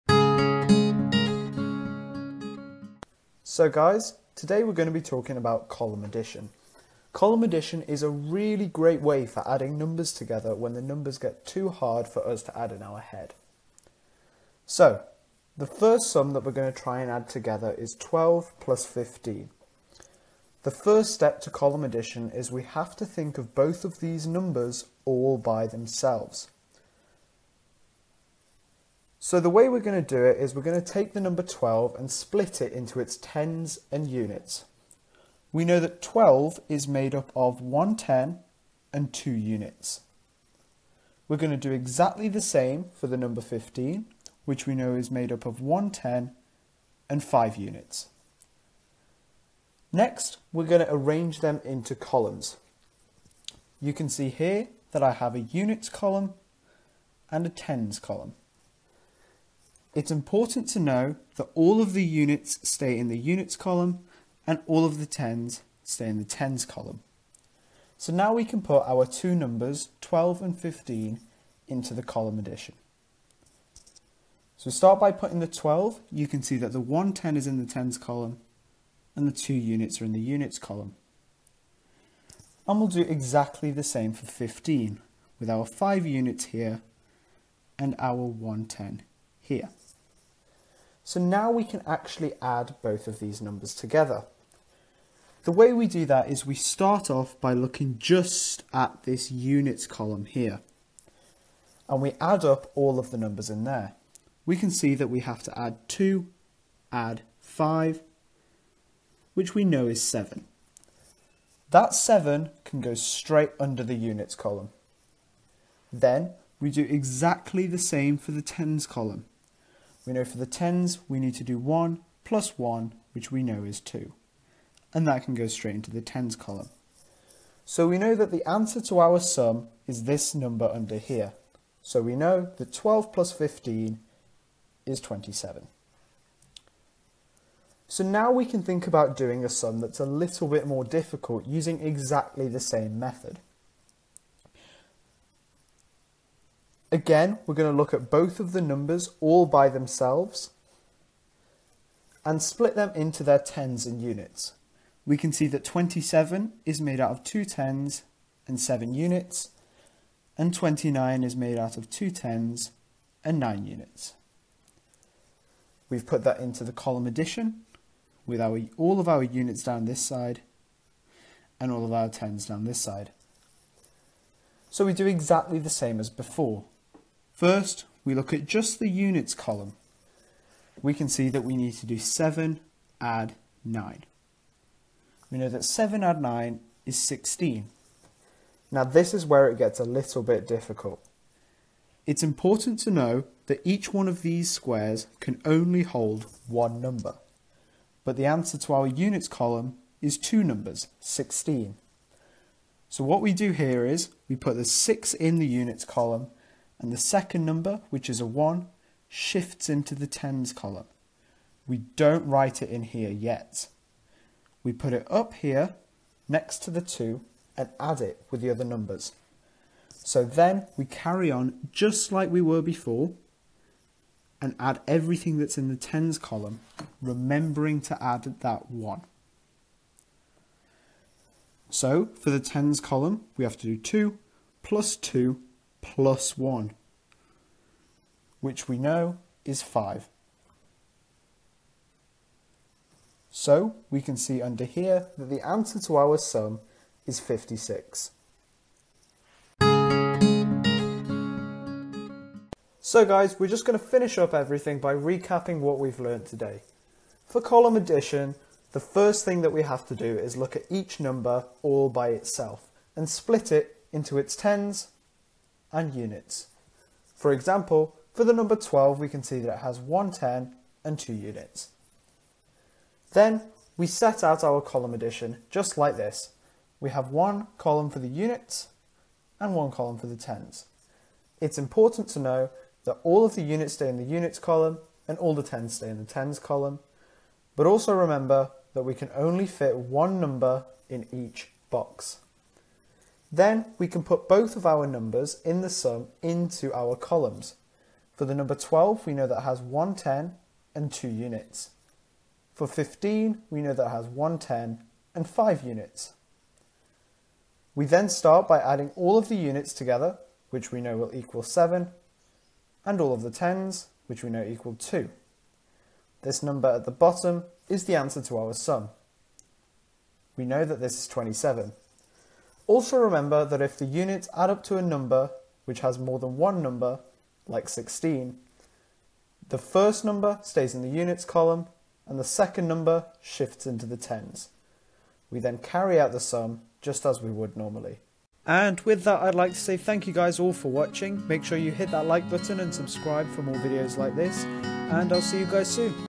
How To Do Column Addition [KS1 MATHS]
How to do basic two-digit column addition with carryover numbers and place value